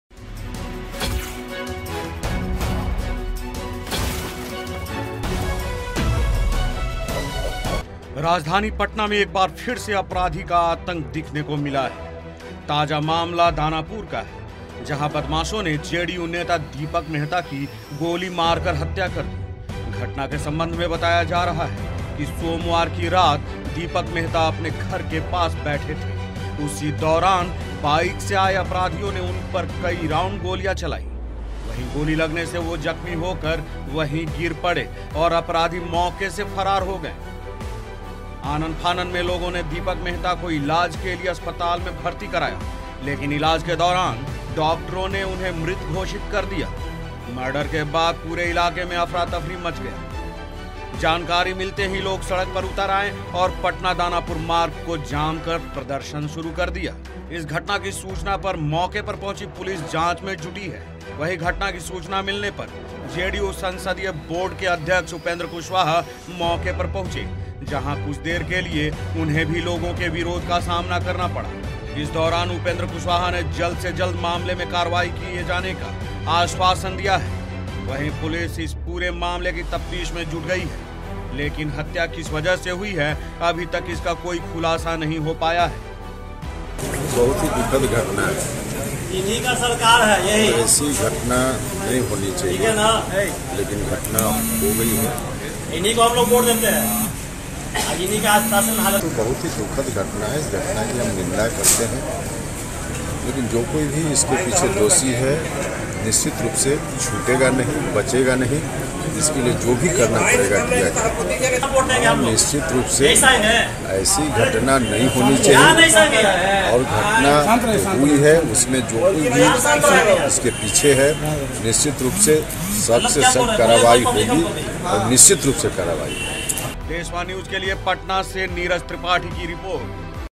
पटना के दानापुर में बड़ी वारदात, एक्शन में पुलिस अपराधियों की तलाश में जुटी है पुलिस
राजधानी पटना में एक बार फिर से अपराधी का आतंक दिखने को मिला है.. ताज़ा मामला दानापुर का है.. जहां बदमाशों ने जेडीयू नेता दीपक मेहता की गोली मारकर हत्या कर दी.. घटना के संबंध में बताया जा रहा है  कि सोमवार की रात दीपक मेहता अपने घर के पास बैठे थे।

A big incident in Danapur, Patna, in action, the police is looking for the criminals. The terror of the criminal has been seen once again in the capital Patna.. The latest case is of Danapur.. where the miscreants shot and killed JDU leader Deepak Mehta. In relation to the incident, it is being told that on Monday night. Deepak Mehta was sitting near his house.